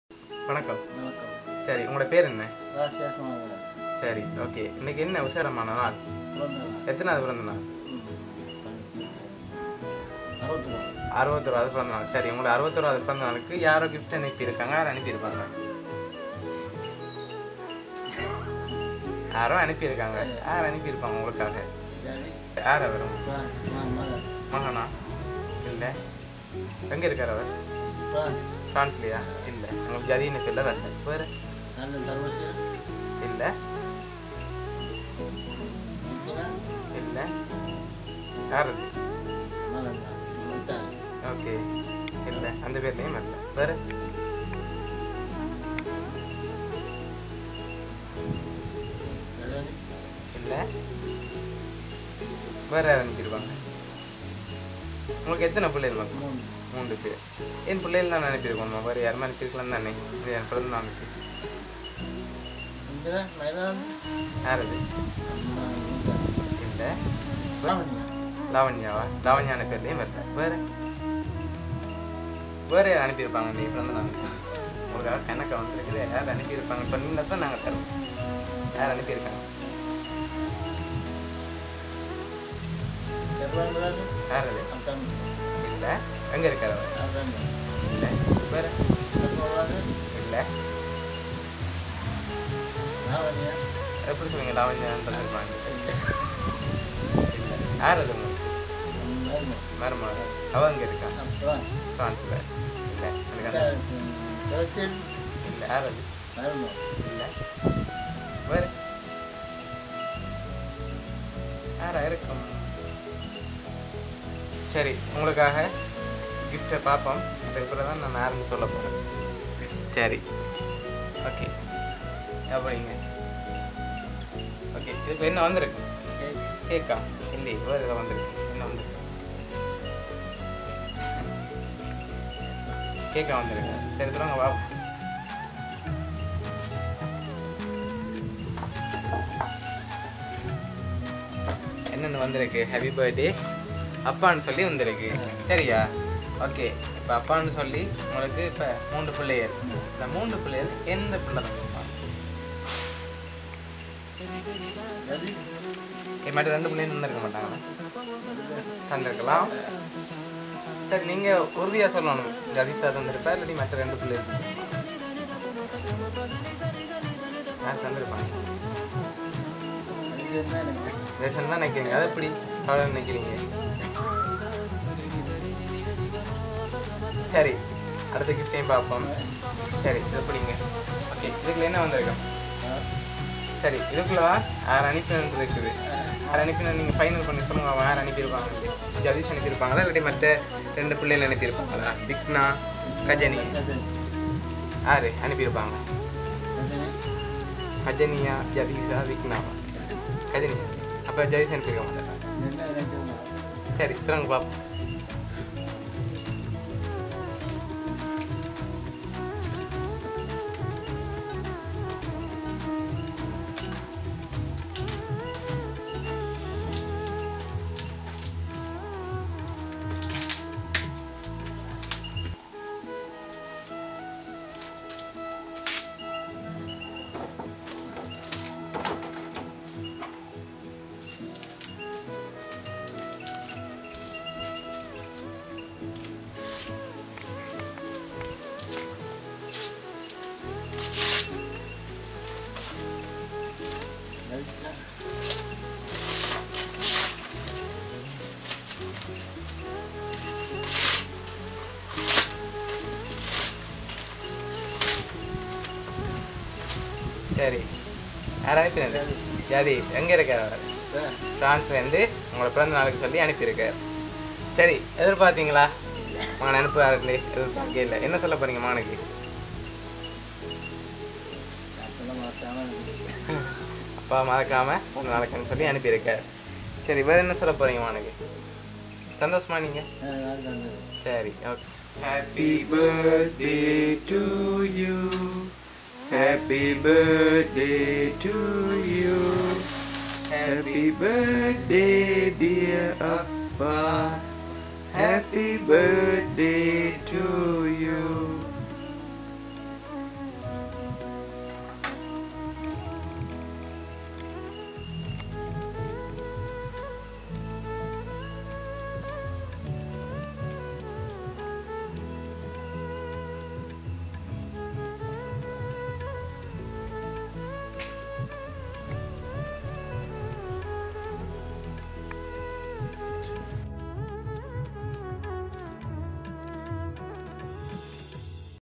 Birthday surprise gifts | Vavuniya Surprise Giftz | Cake | Photo Frame | VSGSQUAD
🎁 Package includes 🎁

🎂 Cake
🖼  Photo Frame

📌 Place - Ukkalankulam, Vavuniya

Thank you so much Dear Valuable Customer Jathees ❤️